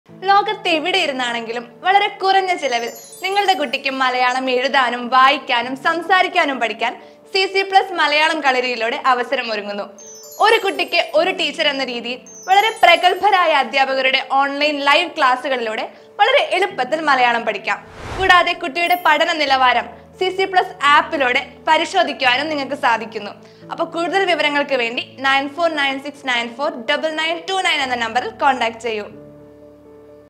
Learn Malayalam With CC Plus Kalari Malayalam Online Coaching | 1 Teacher For A Student
Malayalam Kalari - Malayalam language special training to read & write.

മലയാളം എഴുതാനും വായിക്കാനും അറിയാത്ത കുട്ടികളെ  ലോകത്ത് എവിടെയിരുന്നായാലും കുറഞ്ഞ സമയം കൊണ്ട് വളരെ തുച്ഛമായ ഫീസിൽ പഠിക്കാൻ അവസരം ഒരുക്കുകയാണ് CC Plus മലയാളം കളരിയിലൂടെ . ഈ മികച്ച അവസരത്തിലൂടെ നിങ്ങളുടെ കുട്ടികൾ മലയാളം എഴുതാനും വായിക്കാനും സംസാരിക്കാനും പഠിക്കും.!!!

ഞങ്ങളുടെ പ്രത്യേകതകൾ
• ഓൺലൈൻ ലൈവ് ക്ലാസുകൾ
• പ്രഗൽഭരായ അധ്യാപകർ
• ഒരു കുട്ടിക്ക് ഒരു ടീച്ചർ
• രസകരമായ പ്രവർത്തനങ്ങൾ
• പ്രതിദിന പുരോഗതി റിപ്പോർട്ട്